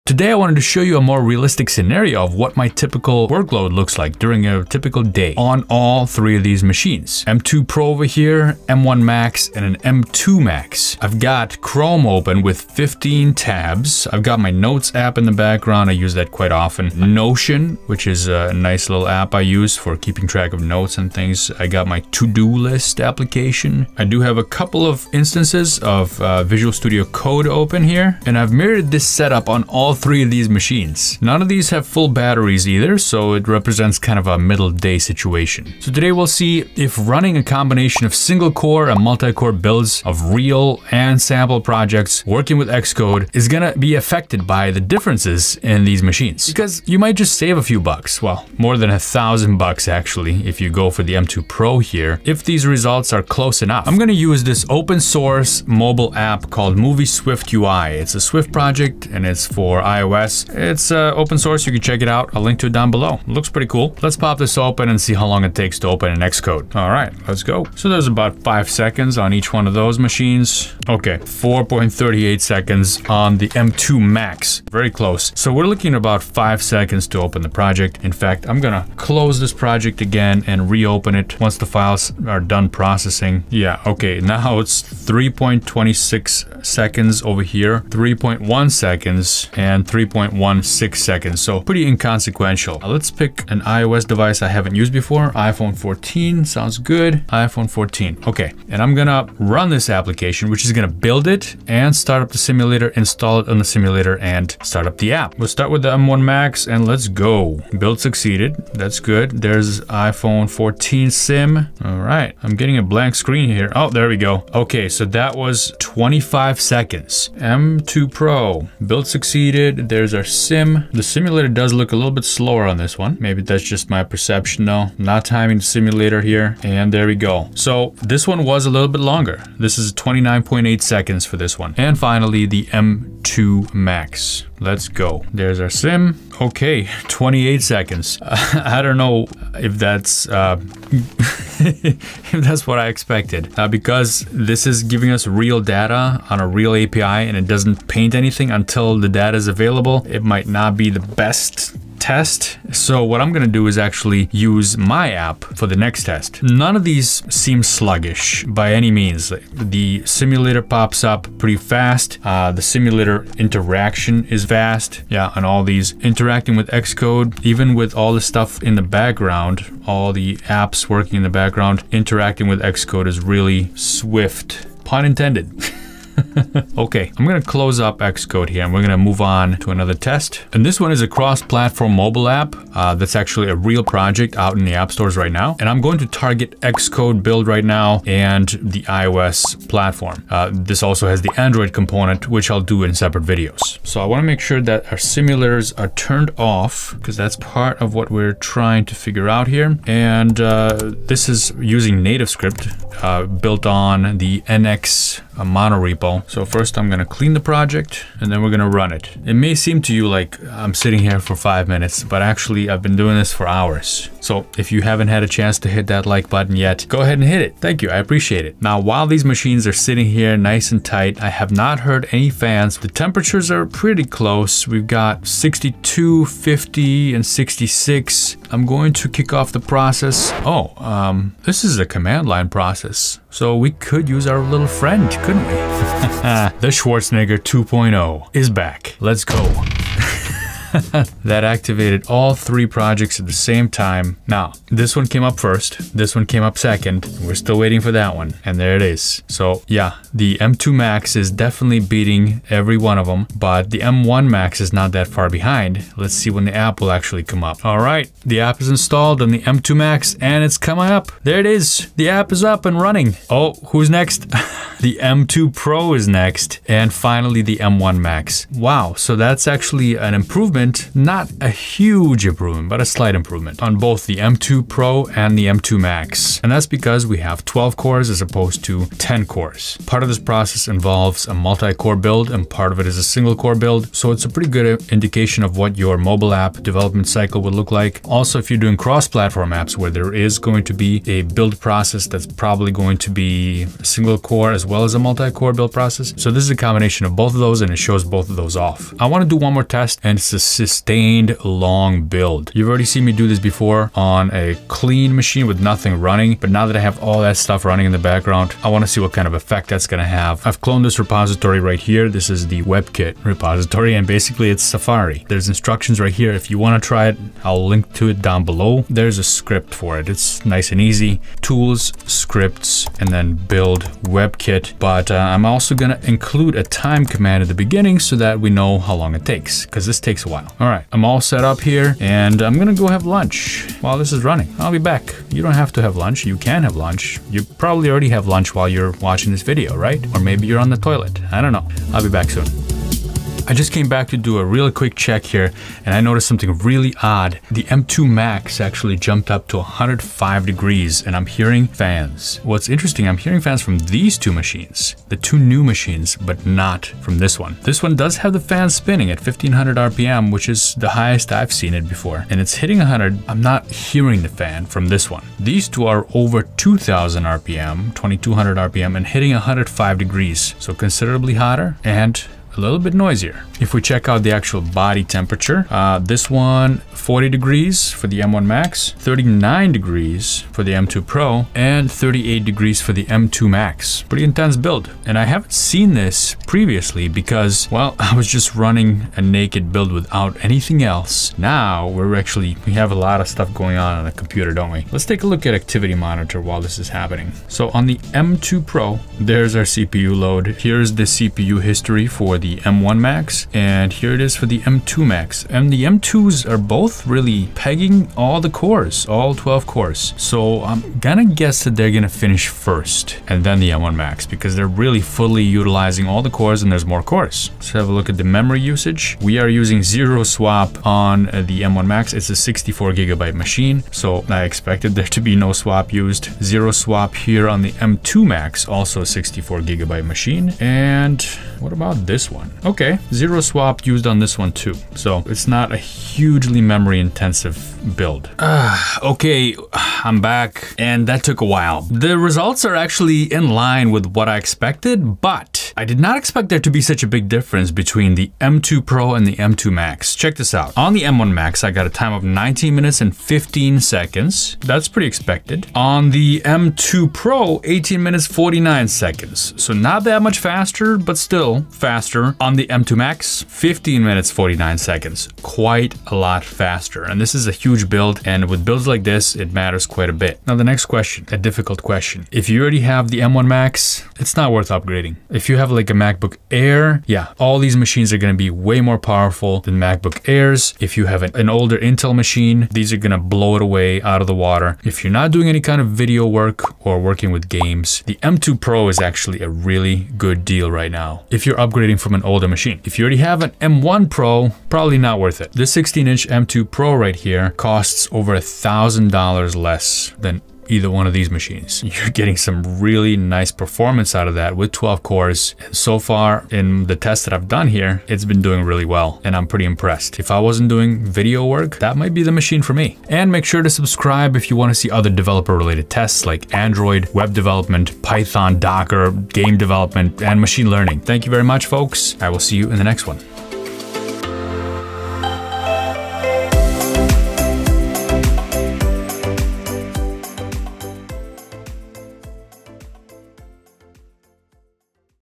Which of these will be my new machine
My software development usability and compile times on three MacBook Pros: M1 Max, M2 Pro, and M2 Max.

👀 *Main gear*
MacBook Pro
LG 27” Monitor

👀 *More gear I use:* (including course creation and youtube stuff)

🏫 FREE COURSES